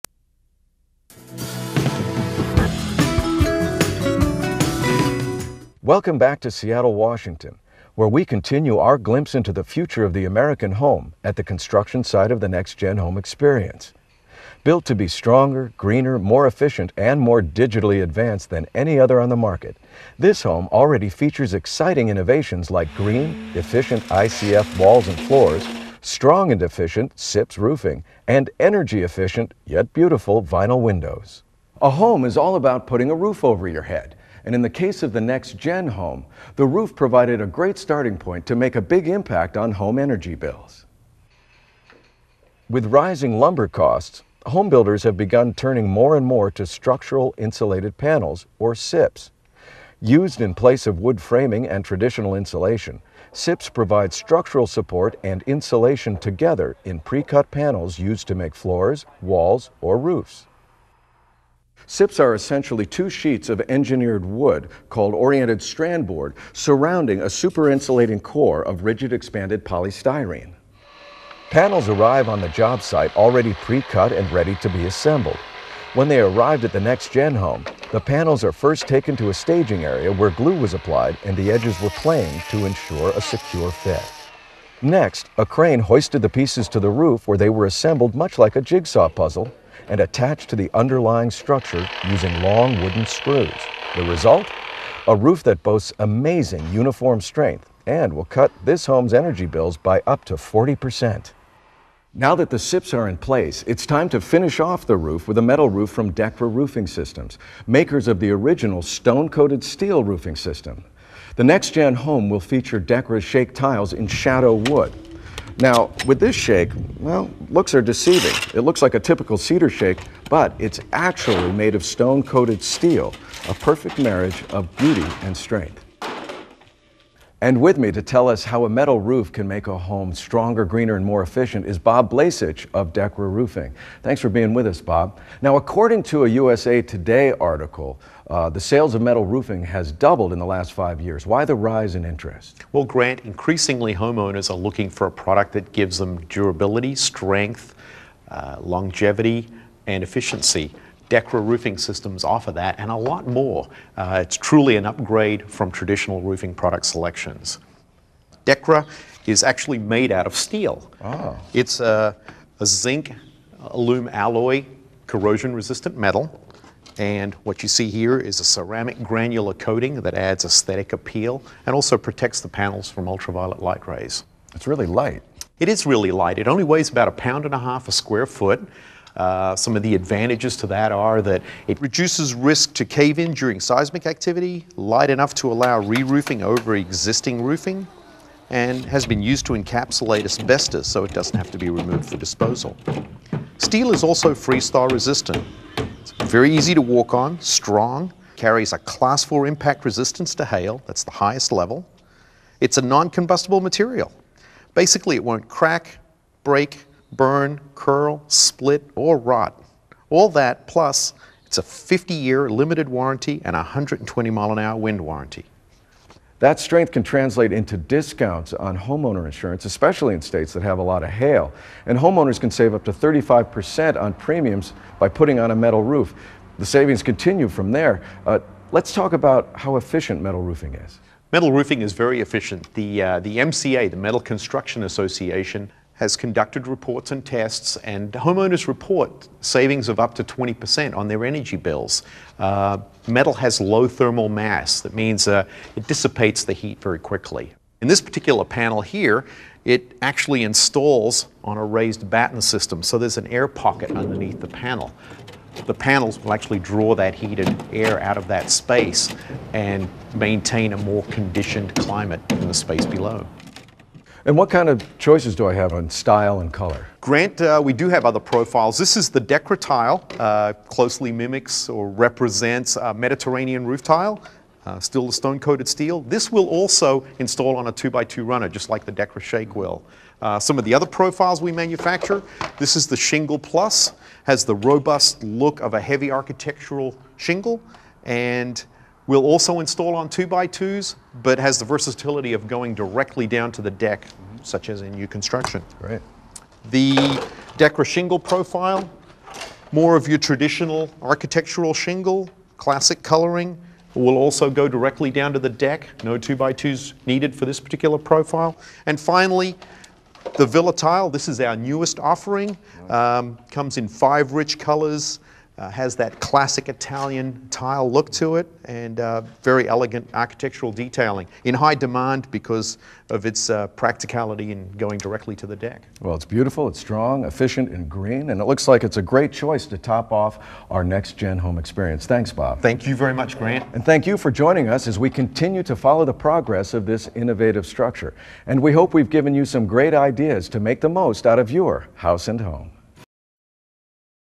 SEATTLE CONCRETE HOME OF THE YEAR
An overview of the walls and roof system of this next generational home in Seattle, built with SIPS, ICF, and Metal Roof Shingles. The home received the award of concrete home of the year.